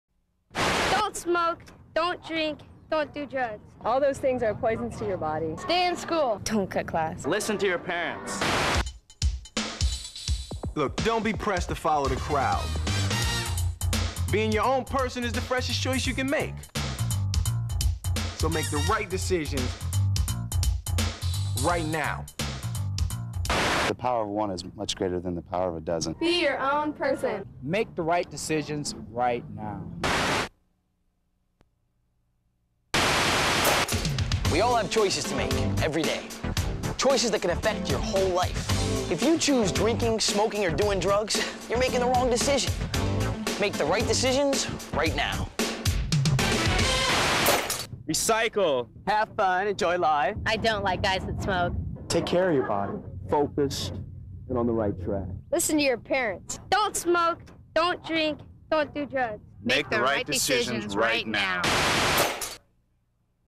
Will Smith Classic PSA
Will Smith (Men in Black / Fresh Prince) and Joey Lawrence star in some classic "Do The Right Thing" anti-drug, anti-smoking PSA's. Directed and produced by Tony Perri.  coronadobeachcompany.com